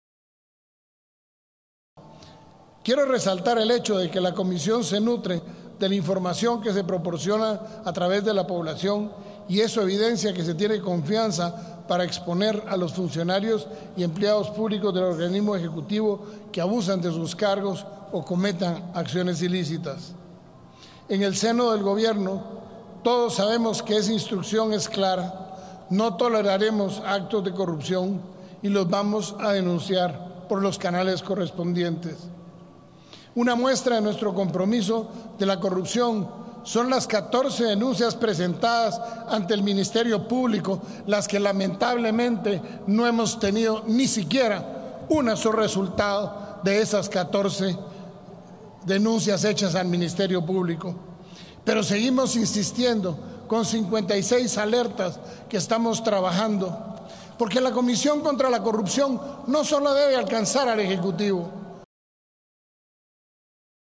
Giammattei pide al MP que accione contra corrupción
En la presentación del informe anual de la comisión contra la Corrupción, el
presidente lamentó el lento accionar del Ministerio Público en los casos por
ellos denunciados, aunque el ente investigador rechazó las sindicaciones e
indicó que sí están trabajando.